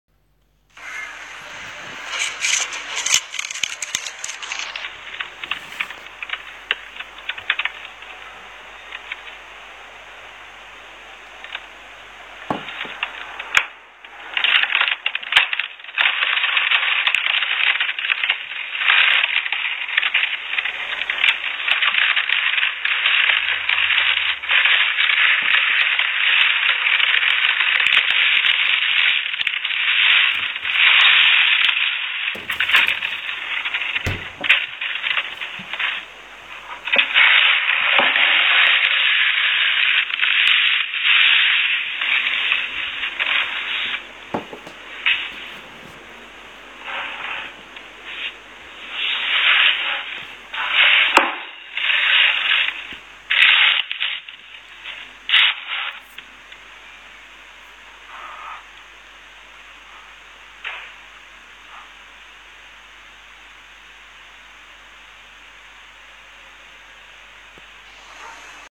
Wasp's Nest Crushing In Slow Motion!
Crushing a used wasp's nest in slow motion